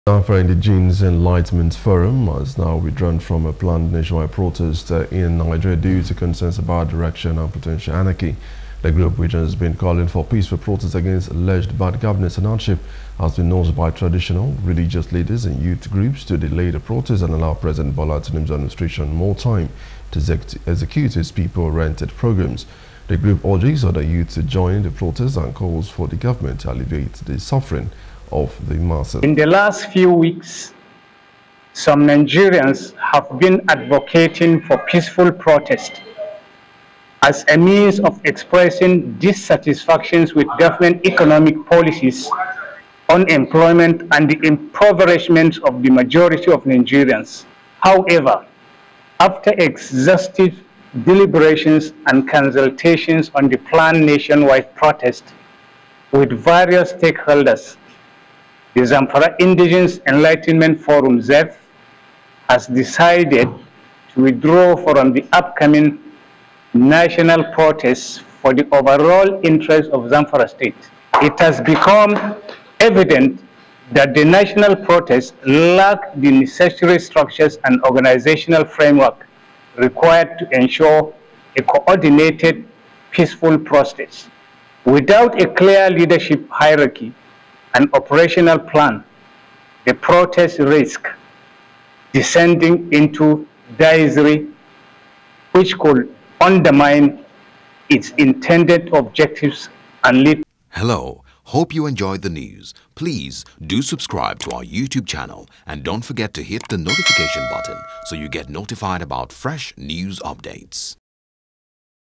Planned Nationwide Protest: Zamfara Indigenes Forum Pulls Out, Says Move Lacks Direction
The Zamfara Indigenes Enlightenment Forum (ZIEF) has withdrawn from a planned nationwide protest in Nigeria due to concerns about direction and potential anarchy. The group, which has been calling for peaceful protests against alleged bad governance and hardship, has been urged by traditional, religious leaders, and youth groups to delay the protests and allow President Bola Tinubu's administration more time to execute his people-oriented programs. The group urges other youth to join the protest and calls for the government to alleviate the suffering of the masses.